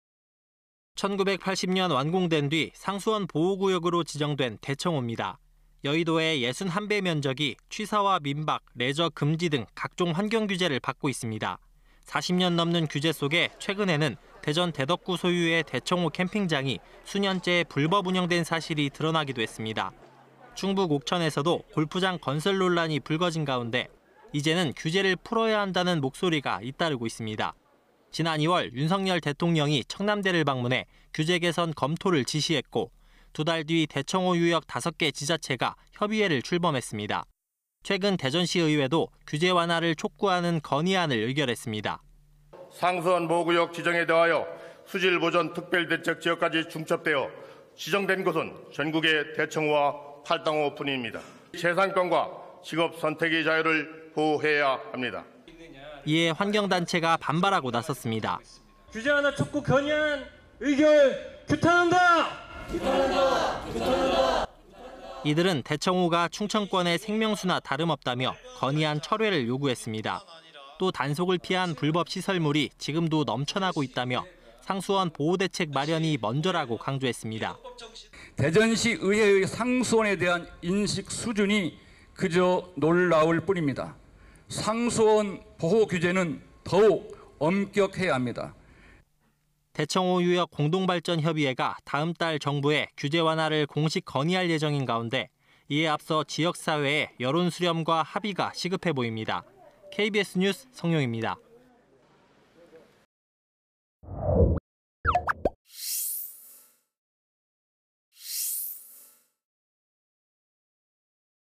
“규제 완화” vs “상수원 보호”…깊어지는 대청호 갈등 / KBS 2023.06.07.
[리포트]

 1980년 완공된 뒤 상수원보호구역으로 지정된 대청호입니다.

 여의도의 61배 면적이 취사와 민박, 레저 금지 등 각종 환경 규제를 받고 있습니다.

 40년 넘는 규제 속에 최근에는 대전 대덕구 소유의 대청호 캠핑장이 수년째 불법 운영된  사실이 드러나기도 했습니다.

 충북 옥천에서도 골프장 건설 논란이 불거진 가운데 이제는 규제를 풀어야 한다는 목소리가 잇따르고 있습니다.

 지난 2월 윤석열 대통령이 청남대를 방문해 규제 개선 검토를 지시했고 두 달 뒤 대청호 유역 5개 지자체가 협의회를 출범했습니다.

 최근 대전시의회도 규제 완화를 촉구하는 건의안을 의결했습니다.

 [송활섭/대전시의원/지난 1일 : "상수원 보호구역 지정에 더해 수질 보전 특별대책 지역까지 중첩돼 지정된 곳은 전국에 대청호와 팔당호뿐입니다. 재산권과 직업선택의 자유를 보호해야 합니다."]

 이에 환경단체가 반발하고 나섰습니다.

["규제 완화 촉구 건의안 의결 규탄한다! 규탄한다!"]

 이들은 대청호가 충청권의 생명수나 다름없다며 건의안 철회를 요구했습니다.

 또 단속을 피한  불법 시설물이 지금도 넘쳐나고 있다며  상수원 보호 대책 마련이 먼저라고 강조했습니다.

 [문성호/대전충남녹색연합 상임대표 : "대전시의회의 상수원에 대한 인식 수준이 그저 놀라울 뿐입니다. 상수원 보호 규제는 더욱 엄격해야 합니다."]

 대청호 유역 공동발전협의회가 다음 달 정부에 규제 완화를 공식 건의할 예정인 가운데 이에 앞서 지역사회의 여론 수렴과 합의가 시급해 보입니다.

 KBS 뉴스 성용희입니다.

 촬영기자:유민철